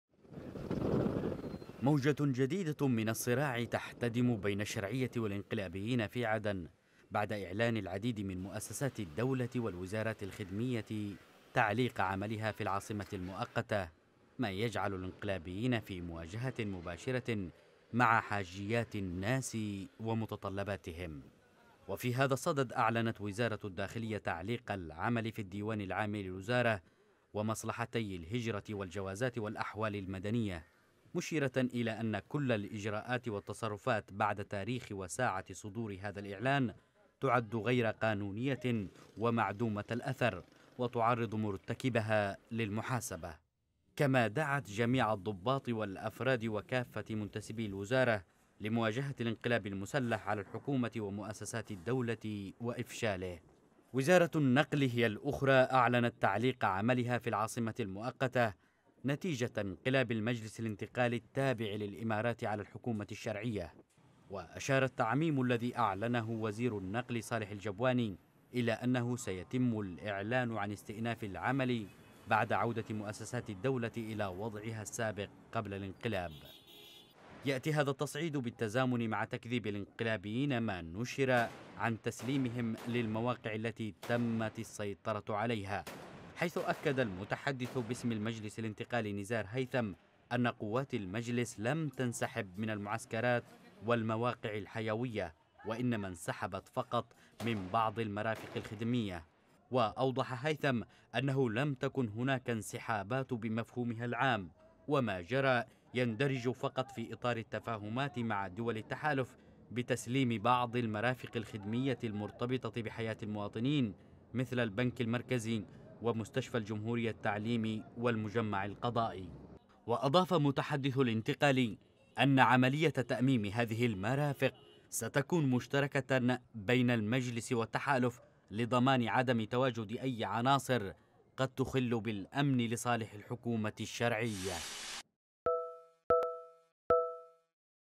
الحكومة تعلق أعمالها في عدن والانقلابيون يرفضون تسليم المواقع التي سيطروا عليها|تقرير:ماهر أبو المجد
قال بيان  للوزارة إن كل الإجراءات والتصرفات بعد تاريخ وساعة صدور هذا الإعلان، تعد غير قانونية ومعدومة الأثر، وتعرّض مرتكبَها للمحاسبة.
هذا وأعلنت وزارتا النقل والخارجية تعليق عملهما في عدن على خلفية سيطرة المجلس الانتقالي على مؤسسات الدولة .
يأتي هذا بالتزامن مع إعلان المجلس الانتقالي التابع للإمارات رفضه الانسحاب من المواقع الحيوية التي سيطر عليها في العاصمة المؤقتة عدن...